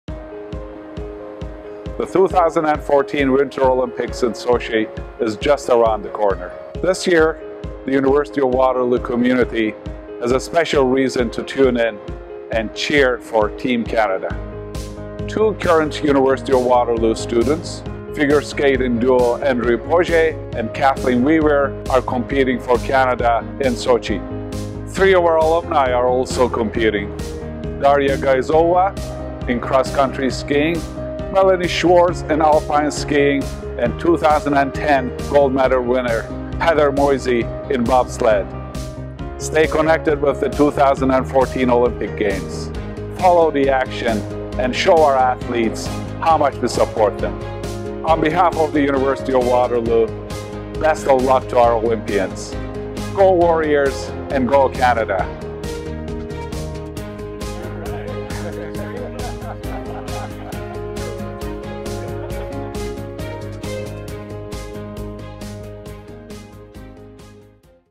Cheer on our SochiWarriors
The University of Waterloo is proud to support our 5 Olympians as they make their way to Sochi for the 2014 Winter Olympics.

Follow the action and send your messages to our proud Warriors by using the SochiWarriors tag on Twitter, Facebook and Instagram!

Go Warriors, Go Canada!